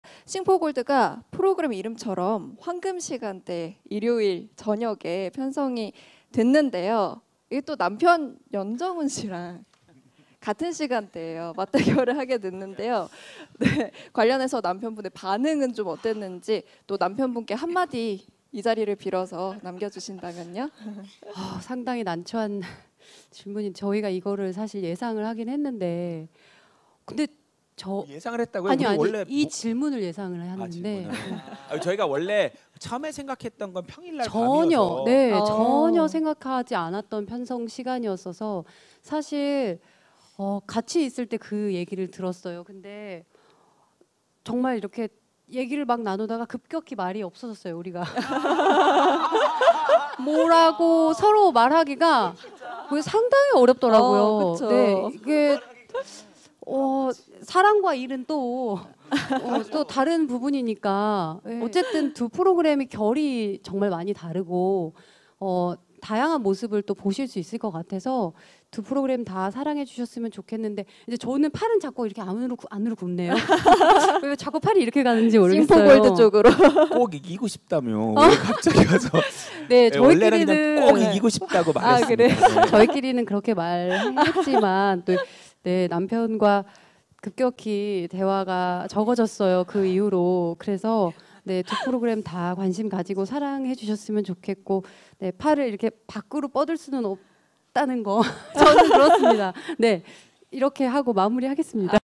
Han Ga in mentioned Youn Junghoon during Sing for Gold online presentation (Sept 23rd)🥰
Han Ga in mentioned Junghoon, her husband and 'competitor'🥰 She said they're talking less with each other since becoming 'competitors' lol - Love and work are 2 different matters😄

*Rough trans, errors are mine*